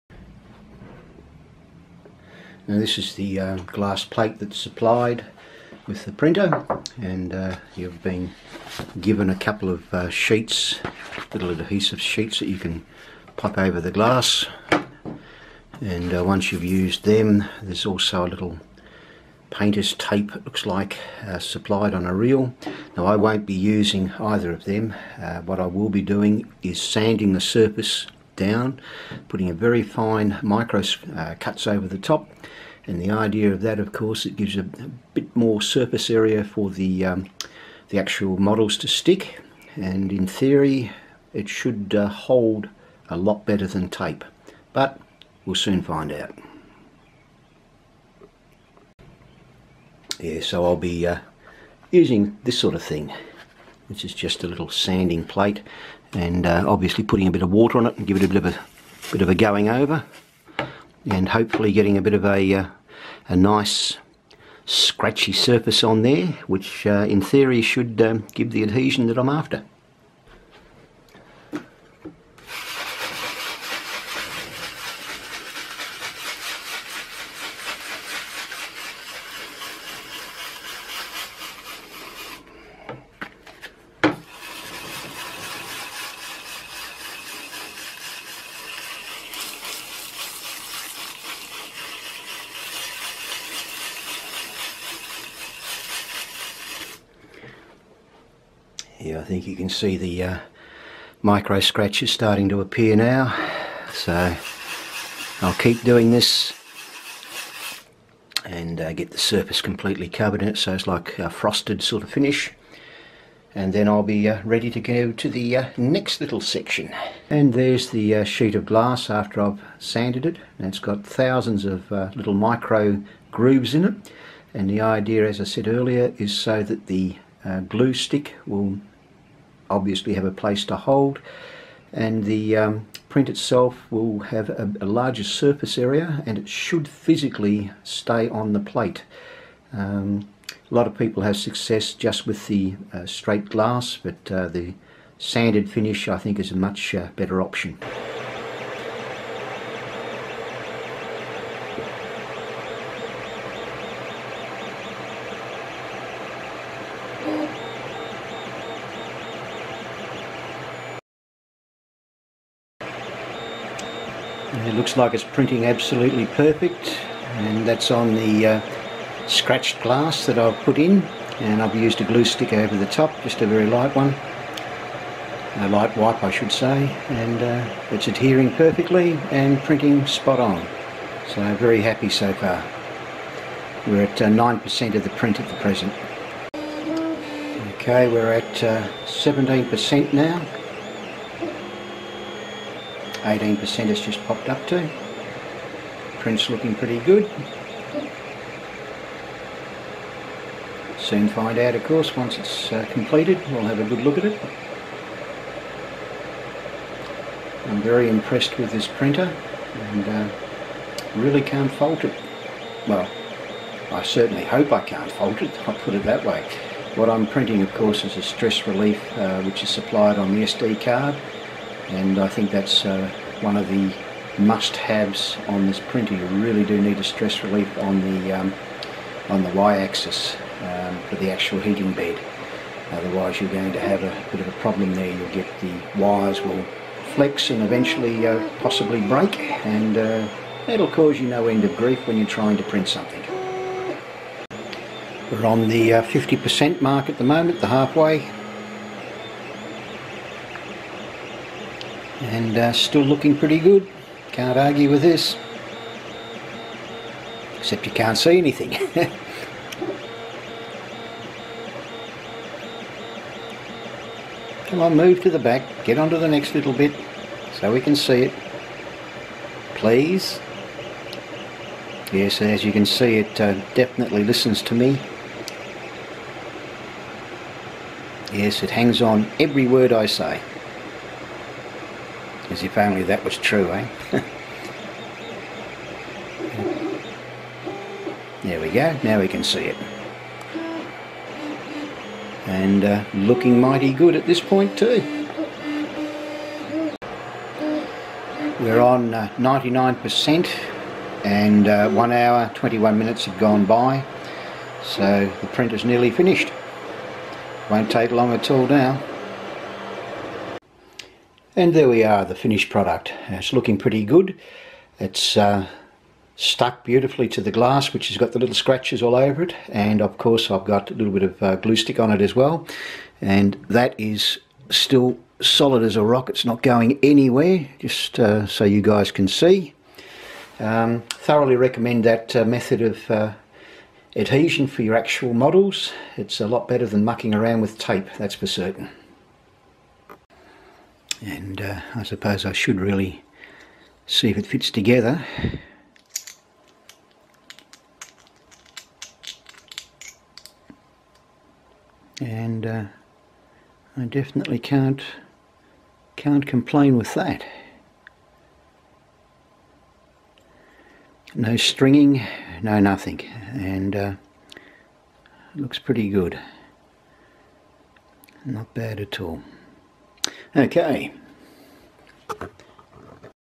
📌 Creality CR-10S 3D Printer Glass Plate Modification for Better 3D Print Adhesion
Creality CR-10S a simple Glass Adhesion solution to improve the quality of your 3D prints.

The prints really stick well to a scratched Glass Surface and make your 3D printing experience a lot better.

📌 Mine came directly from the Creality Original Factory.
They are really good to deal with, and always have the latest Creality products,

I have found that by simply scratching the surface of the glass really improves adhesion.
The best bit is it is easy to do and won't break the bank.
BONUS: If you don't like this mod you simply turn the glass over and print on the smooth side, it's a win win all rounder.

My next Video will be about setting the print bed to the correct level.